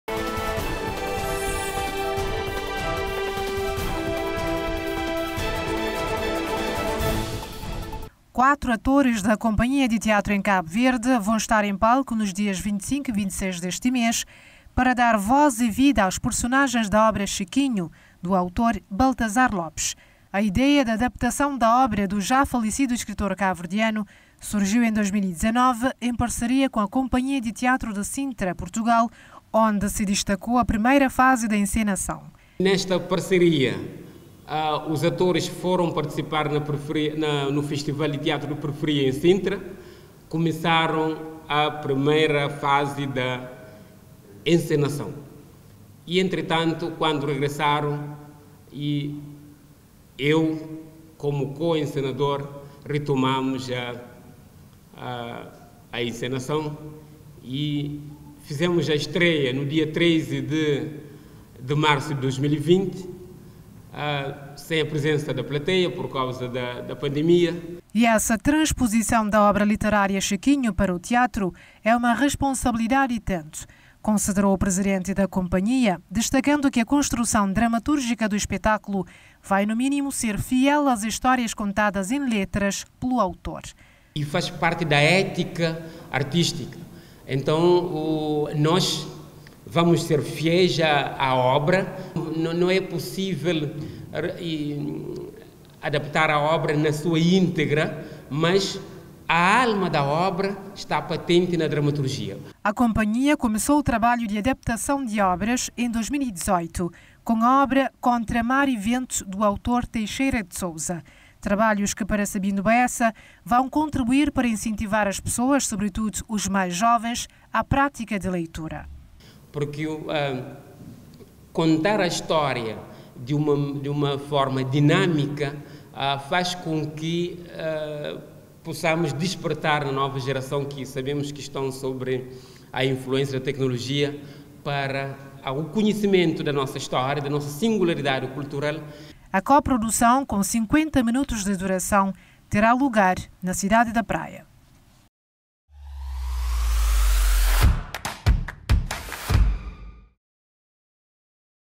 PEÇA TEATRAL INSPIRADA EM OBRA DE CHIQUINHO
O espetáculo conta com a apresentação de quatro atores cabo-verdiano e promete fidelidade aos conteúdos da obra, conforme o porta-voz do elenco.
👇 Acompanhe também👇📌